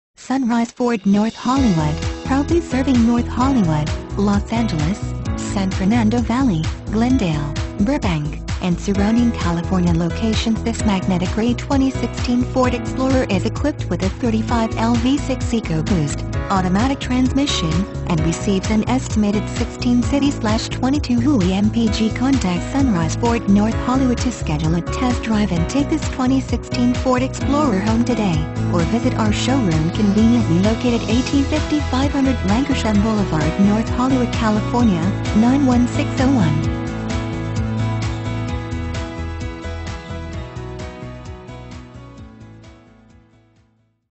2016 Ford Explorer North Hollywood,Los Angeles,San Fernando Valley,Glendale,Burbank M61068T
2016 Ford Explorer 4WD 4dr Platinum

5500 Lankershim Blvd.

SUNRISE FORD NORTH HOLLYWOOD, PROUDLY SERVING NORTH HOLLYWOOD, LOS ANGELES, SAN FERNANDO VALLEY, GLENDALE, BURBANK, AND SURROUNDING CALIFORNIA LOCATIONS THIS MAGNETIC GRAY 2016 Ford EXPLORER IS EQUIPPED WITH A 3.5L V6 EcoBoost, AUTOMATIC TRANSMISSION, AND RECEIVES AN ESTIMATED 16 City/22 Hwy MPG. NORTH HOLLYWOOD CALIFORNIA 91601.